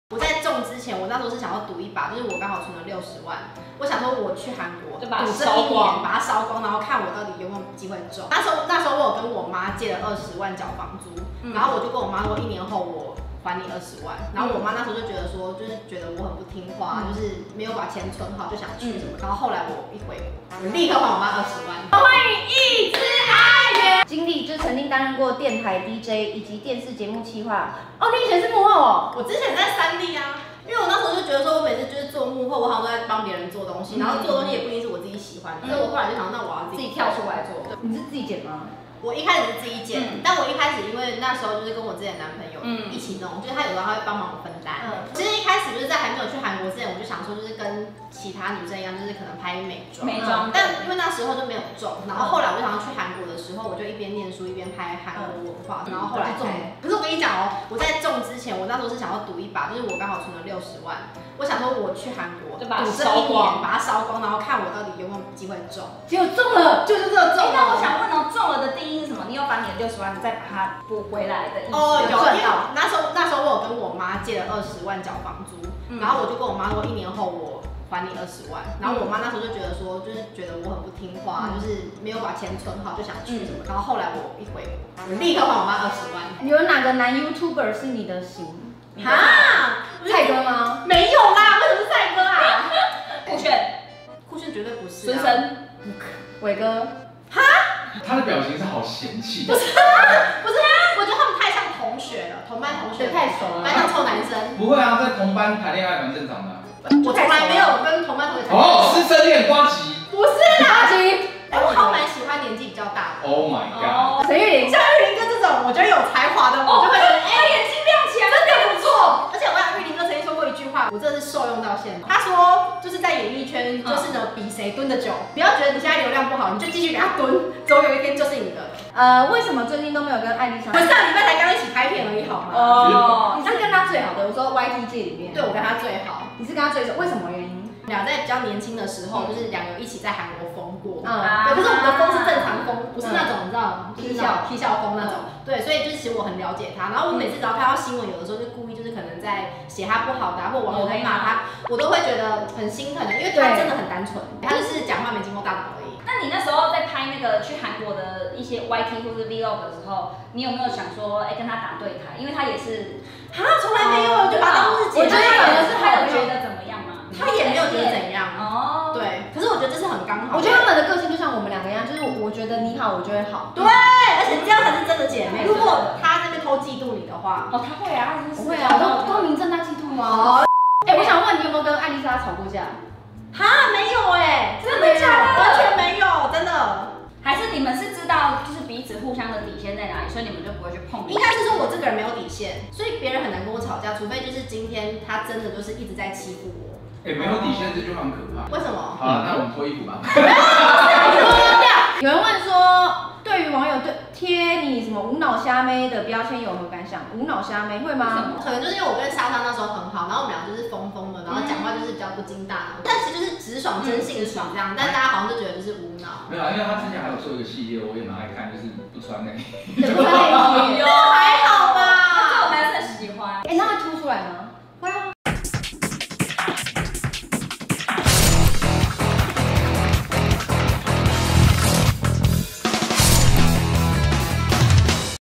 一隻阿圓赴韓國燒60萬積蓄 向媽媽借20萬被狂唸 一年後霸氣還款 @linda-egg ｜94要賺錢
@linda-egg 授權